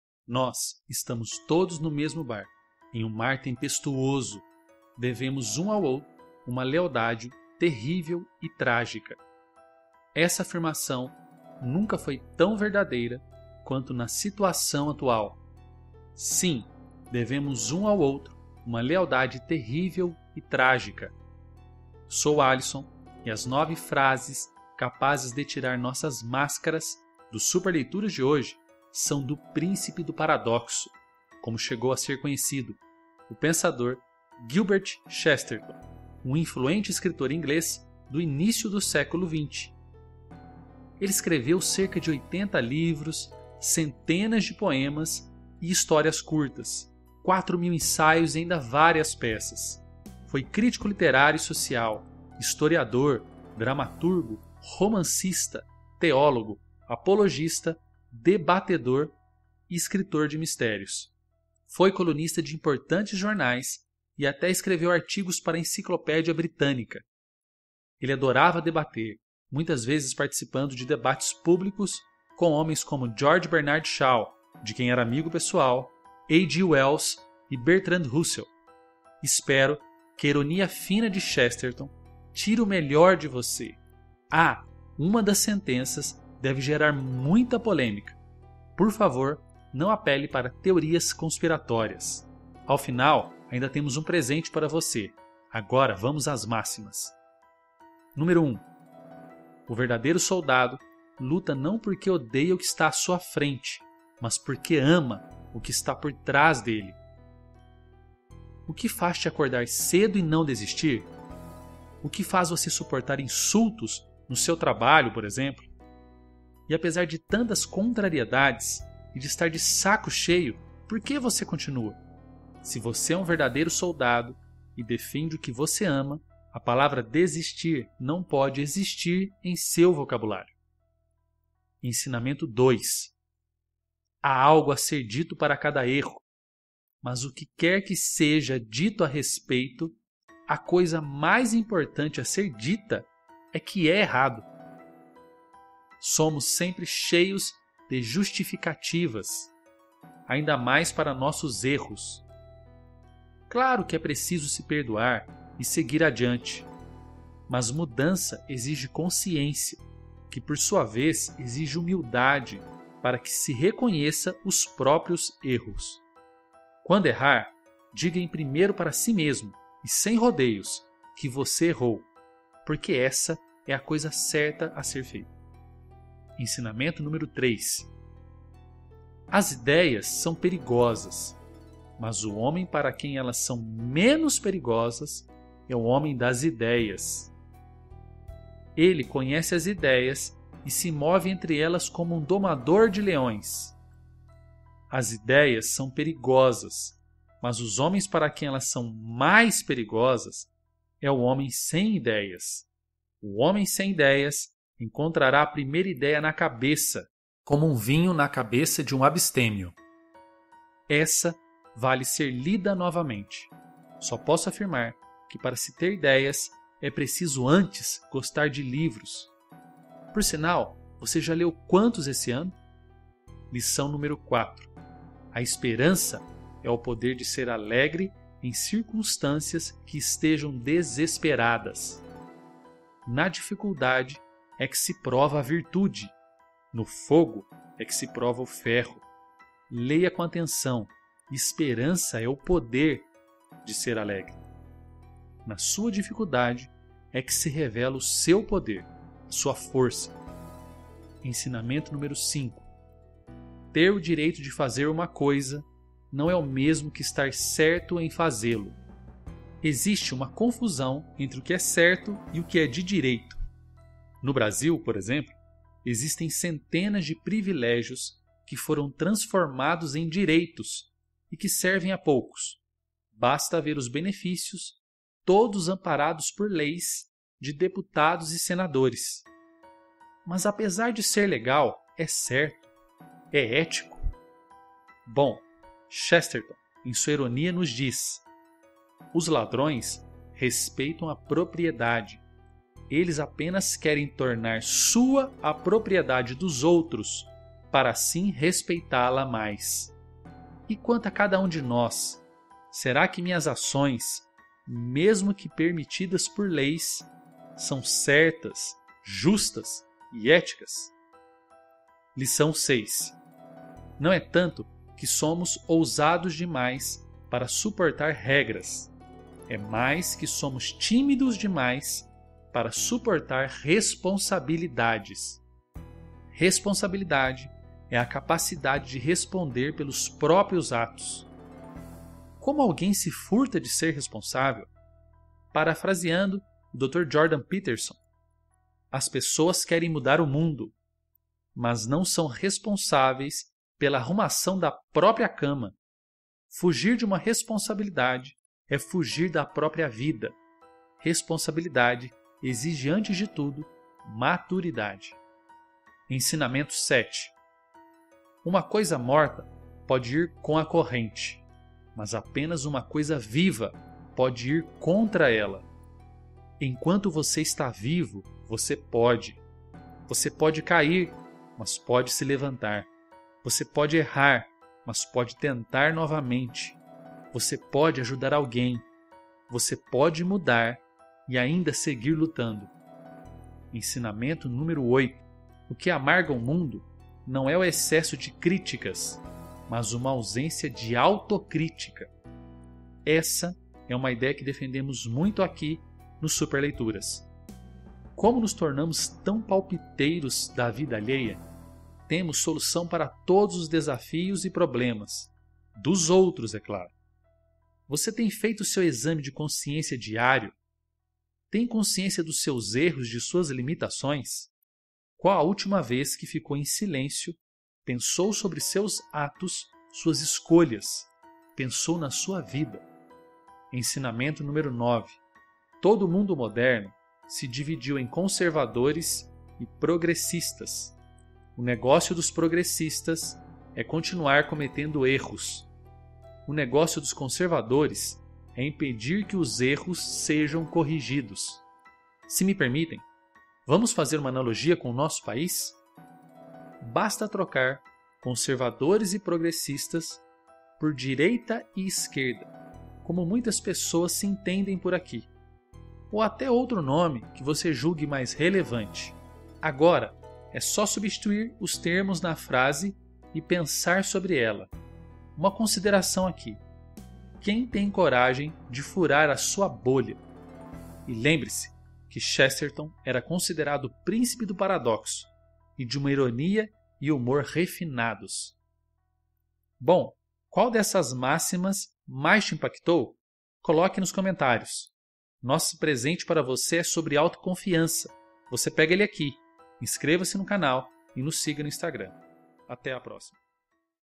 9 Lições de Chesterton | Filosofia de vida | Literatura
Como ser transformado pela Filosofia na Prática:
Participe dos nossos grupos de comunicação:
O pensador Gilbert Keith Chesterton um influente escritor inglês do início do século XX. Um gigante da literatura universal e com uma Filosofia de vida inspiradora.

Escreveu cerca de 80 livros,  centenas de poemas, cerca de duzentas histórias curtas, quatro mil ensaios e várias peças. Foi crítico literário e social, historiador, dramaturgo, romancista, teólogo, apologista, debatedor e escritor de mistérios. A coletânea de histórias de seu detetive o Padre Brown virou até série de TV.

 Foi colunista de importantes jornais e até escreveu artigos para a Enciclopédia Britânica.

Ele adorava debater, muitas vezes participando de debates públicos ​​com homens como George Bernard Shaw (de quem era amigo pessoal) , HG Wells  e Bertrand Russell.

Espero que a ironia fina de Chesterton tire o melhor de você.